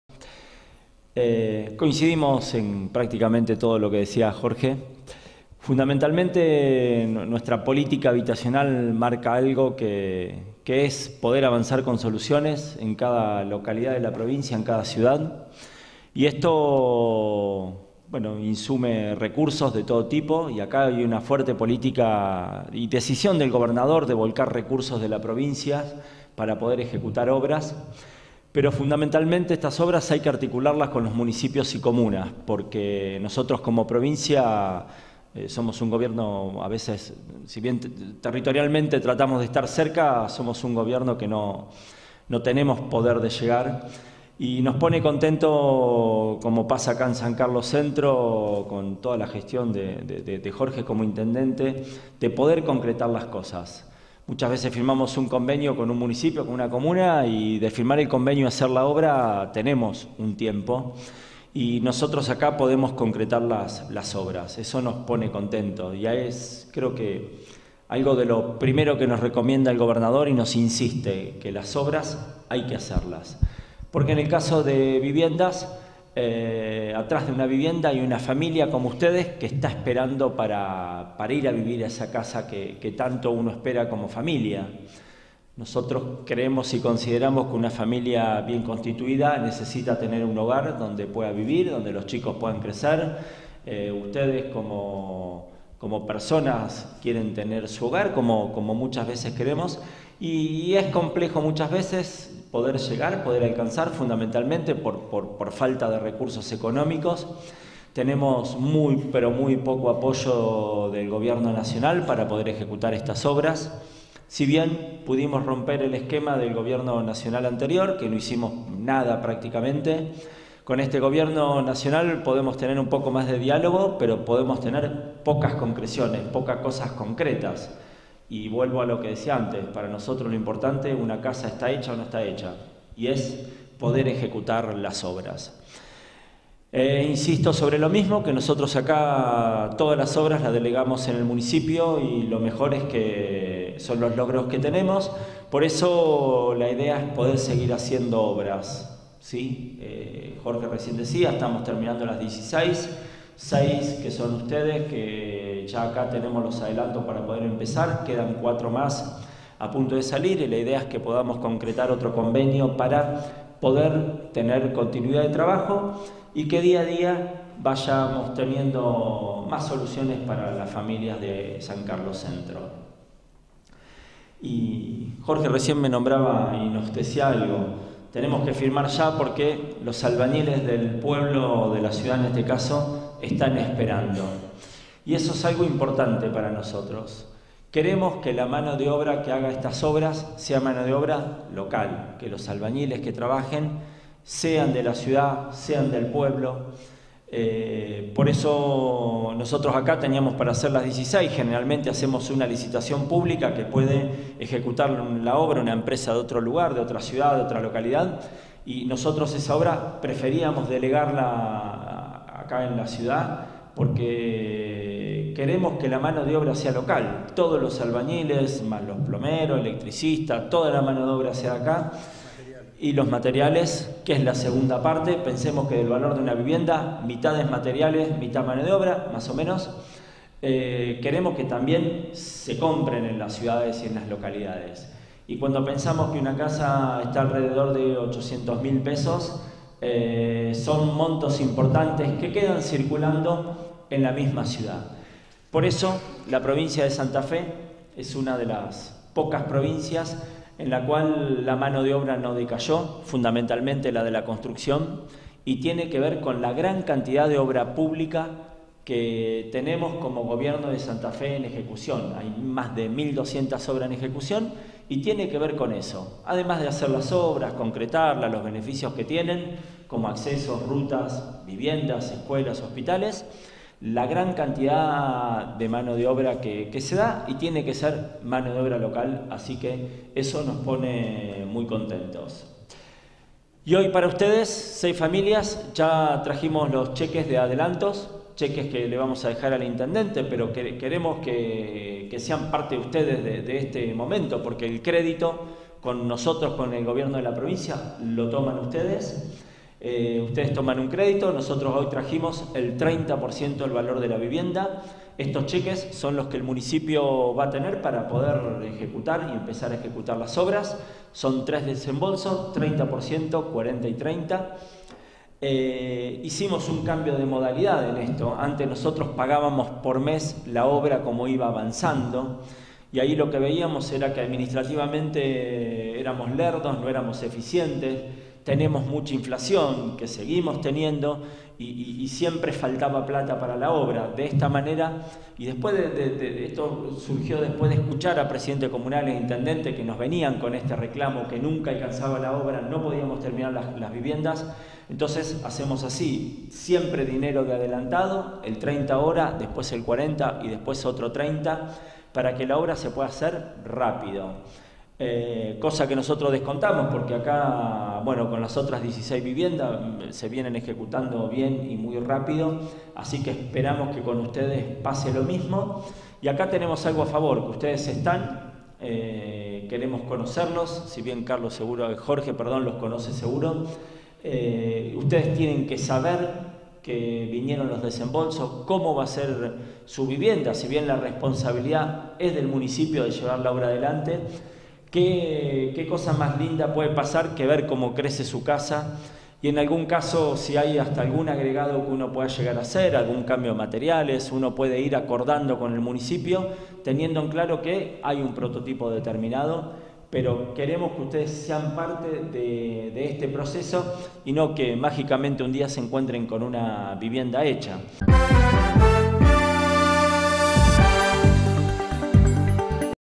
Diego Leone - Secretario de Hábitat
Firma de convenio para la construcción de 6 viviendas.